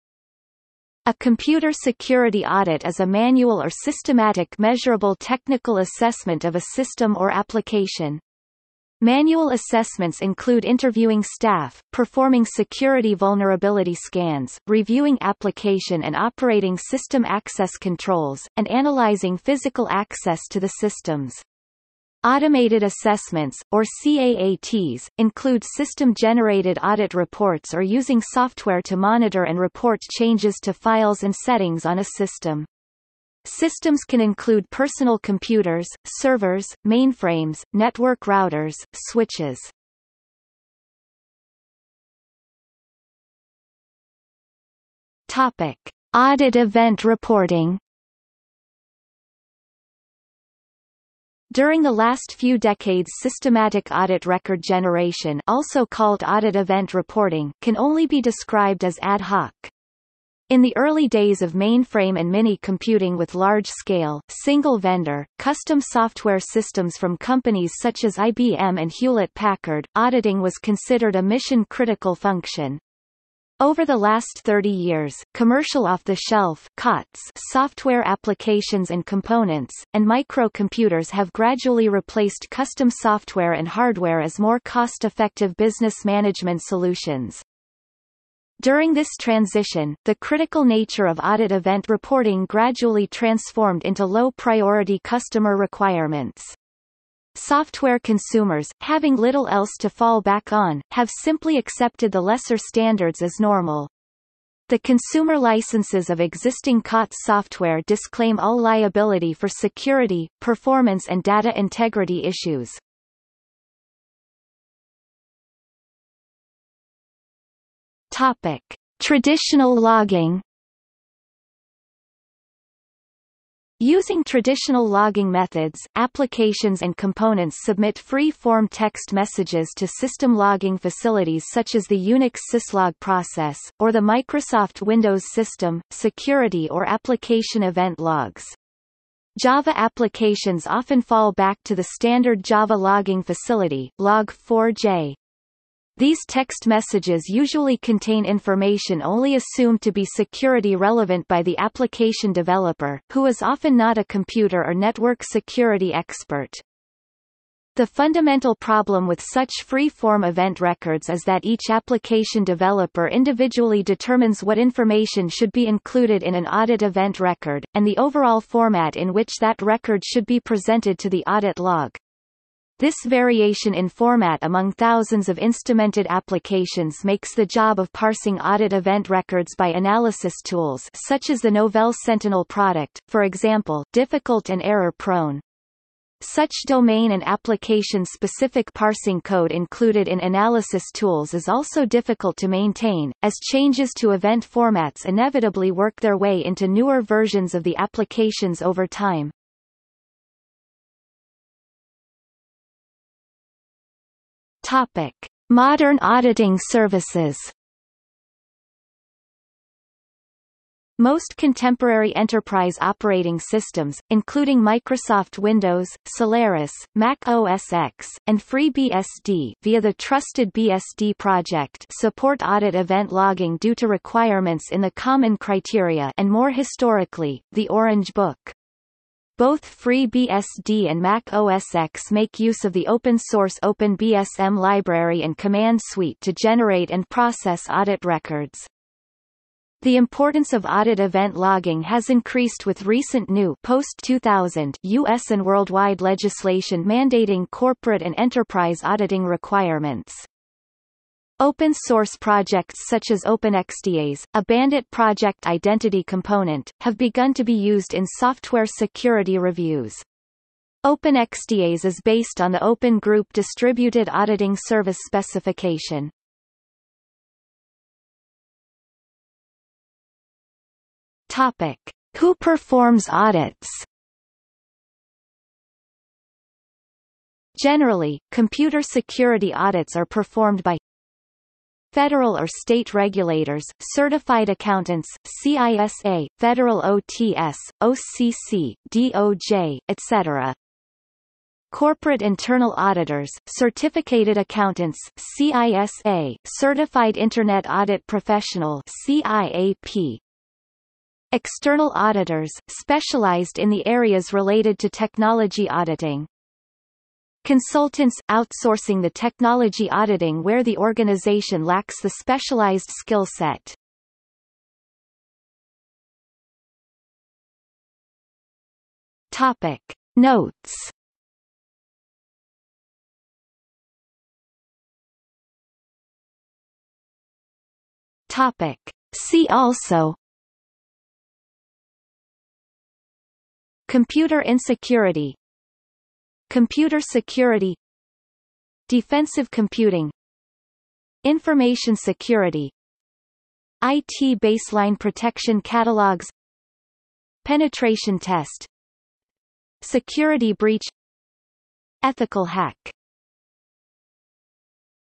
Information technology security audit | Wikipedia audio article
00:00:41 1 Audit event reporting
00:01:54 1.1 Traditional Logging
00:03:21 1.2 Modern Auditing Services
00:04:27 2 Who performs audits
00:05:15 3 Notes
00:05:24 4 See also

- Socrates

SUMMARY
A computer security audit is a manual or systematic measurable technical assessment of a system or application. Manual assessments include interviewing staff, performing security vulnerability scans, reviewing application and operating system access controls, and analyzing physical access to the systems.  Automated assessments, or CAAT's, include system generated audit reports or using software to monitor and report changes to files and settings on a system.  Systems can include personal computers, servers, mainframes, network routers, switches.